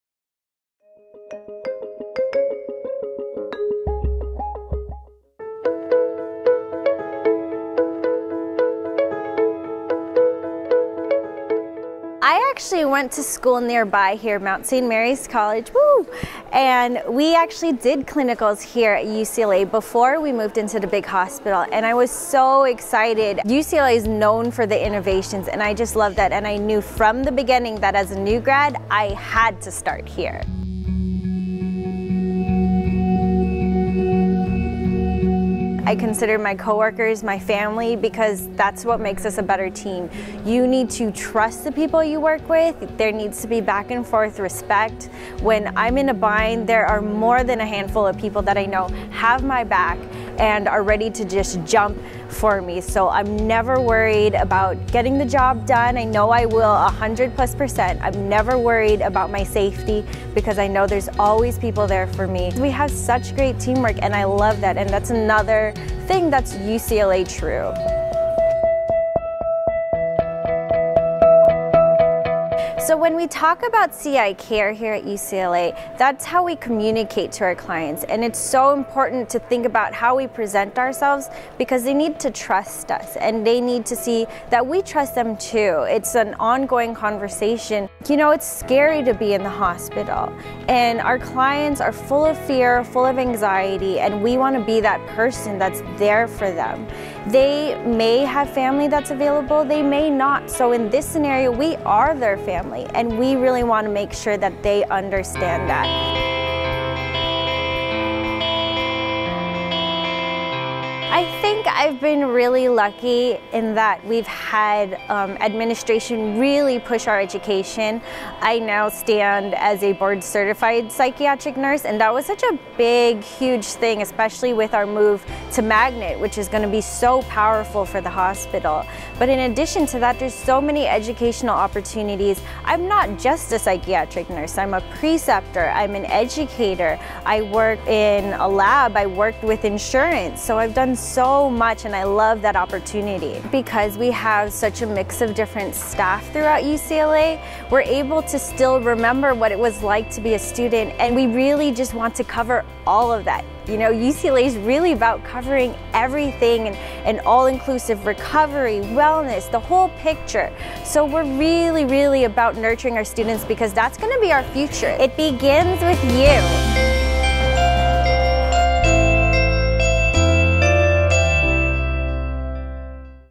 Grace - Clinical Nurse, Resnick Neuropsychiatric Hospital
As a nursing student at Mount St. Mary’s College in Los Angeles, Grace did her clinicals at UCLA. She knew that this was where she wanted to begin her nursing career. Today, she is a Board Certified Psychiatric Nurse working in the Resnick Neuropsychiatric Hospital at UCLA.